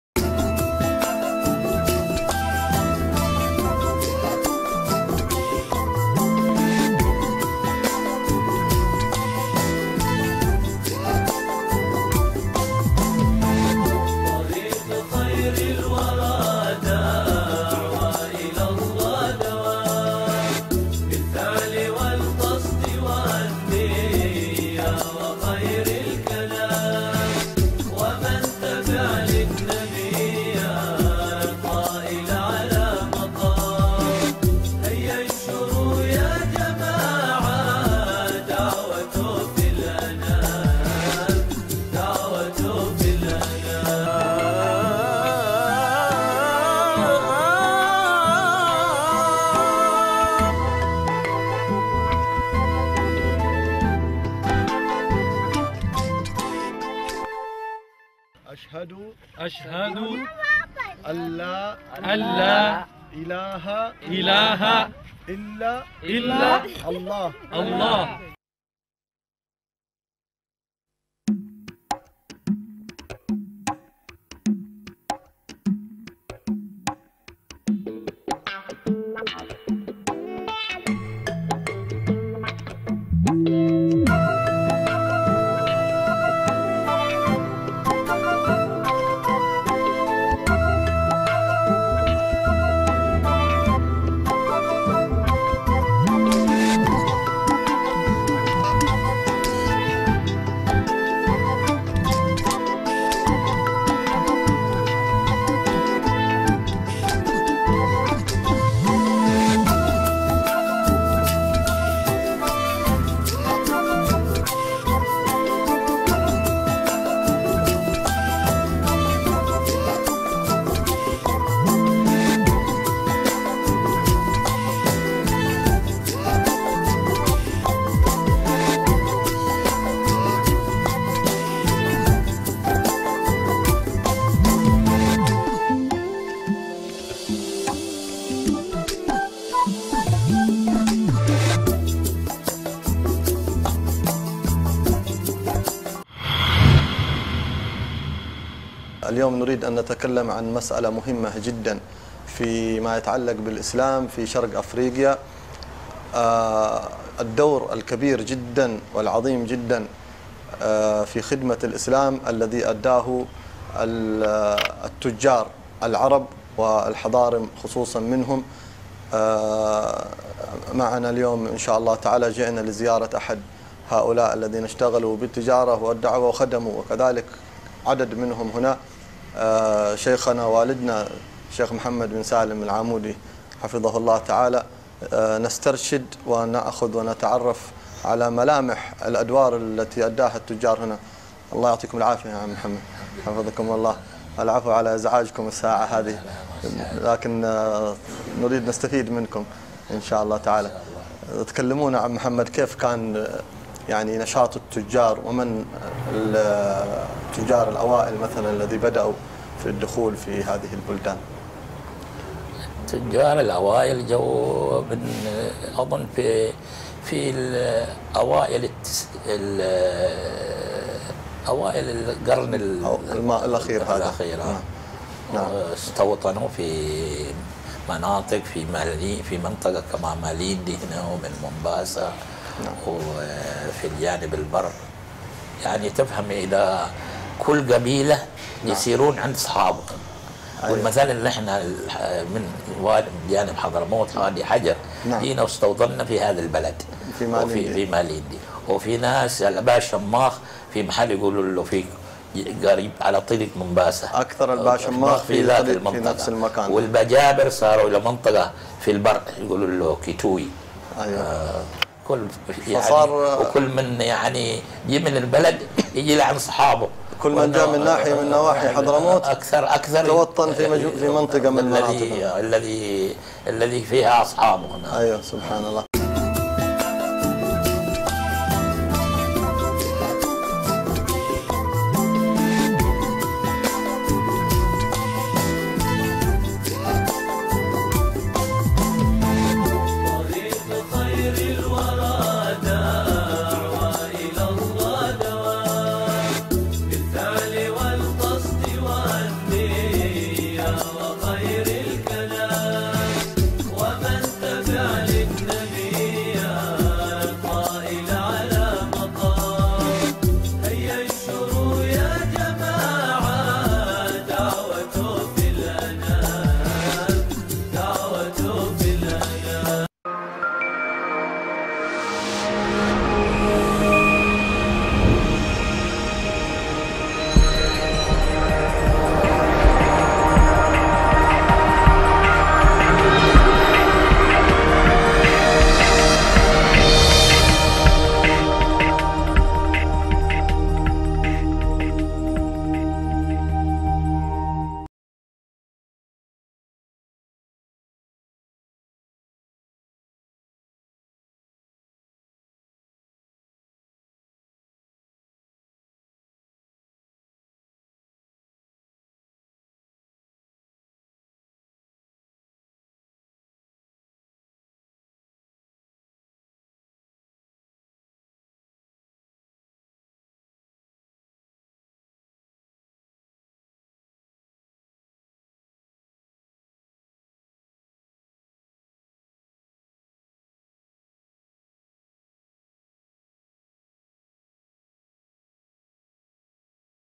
Saksikan NABAWI TV Perjalanan Afrika
Perjalanan dakwah Alhabib Muhammad Assegaf di negeri afrika